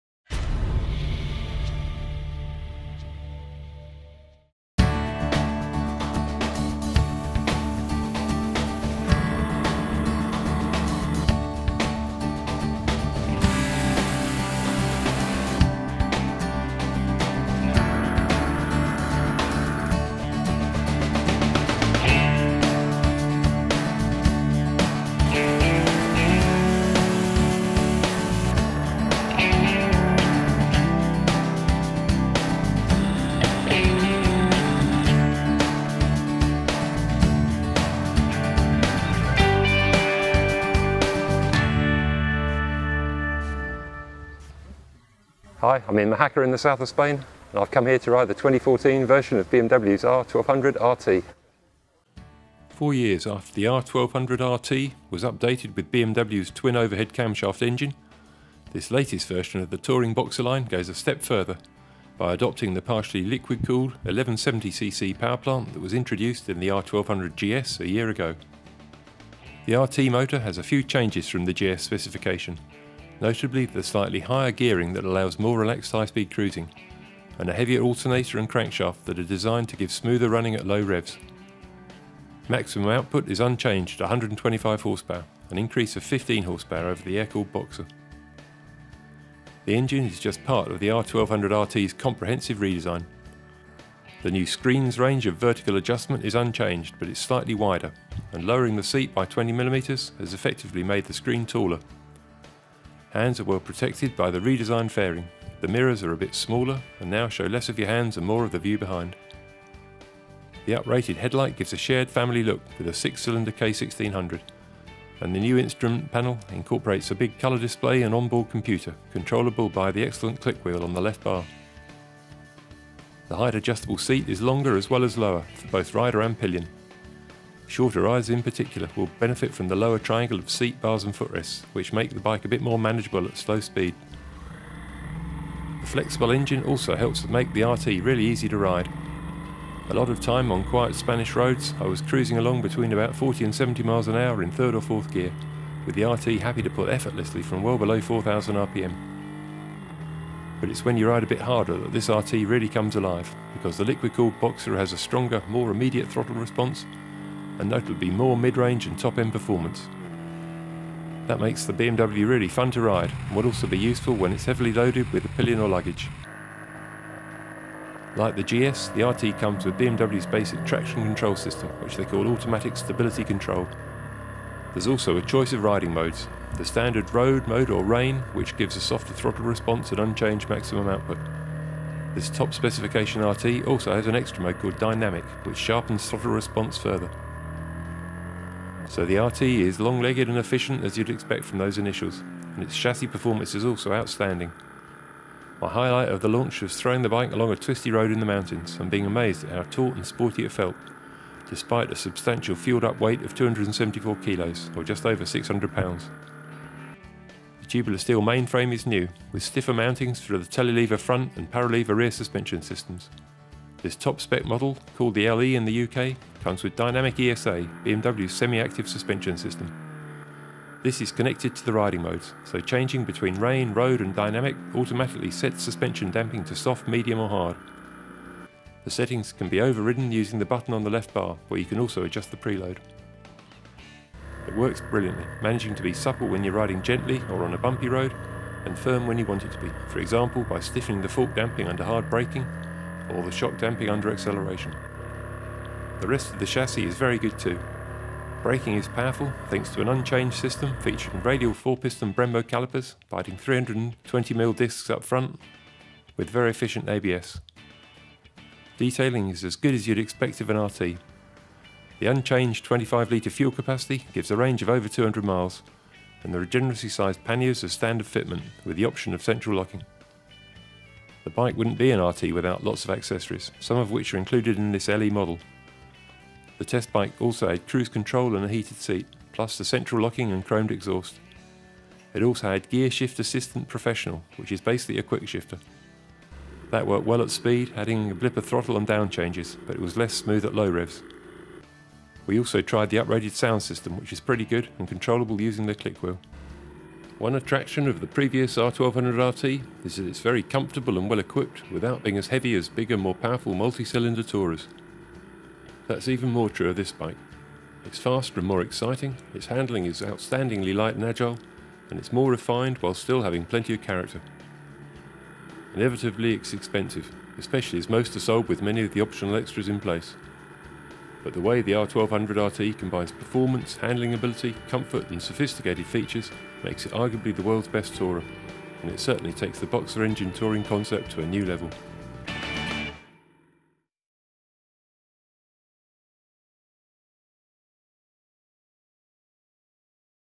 BMW R1200RT 2014 launch test review
Motorcycle journalist Roland Brown tests BMW's R1200RT tourer on the liquid-cooled boxer's international press launch in Spain